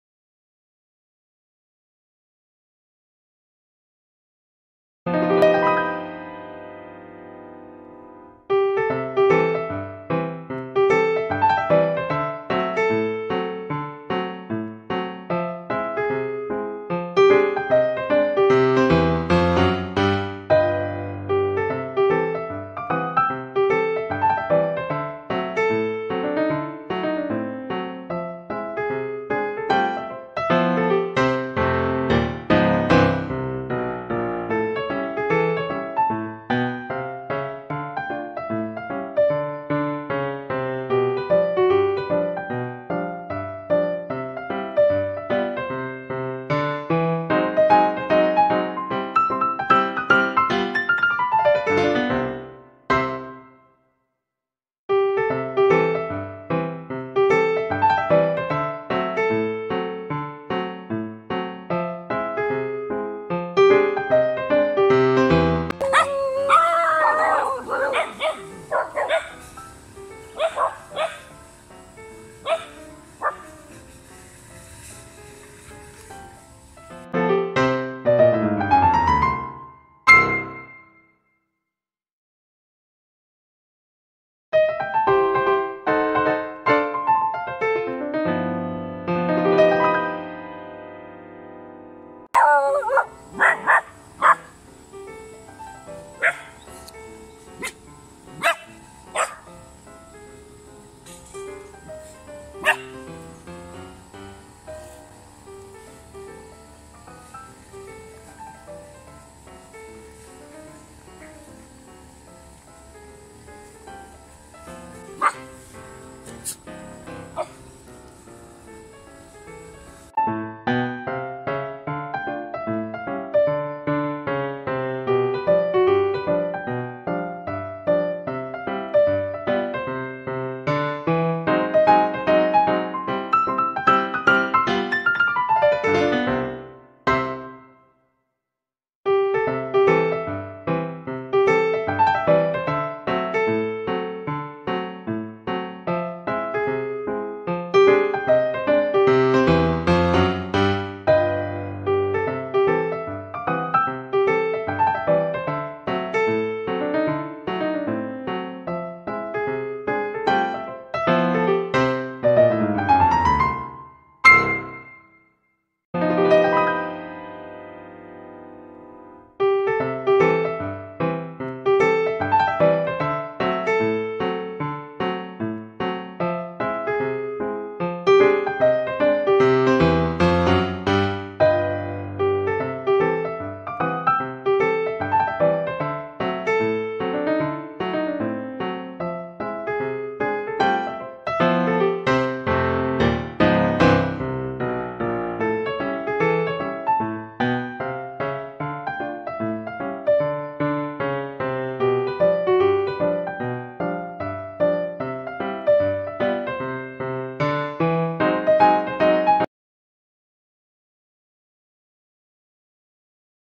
ANJING EKOR PENDEK ANAK NINJA??! KENALAN DULU SAMA SI KEMBAR
Kenalan dulu sama si kembar ekor pendek, anak anjing tapi kayak ninja kelakuannya.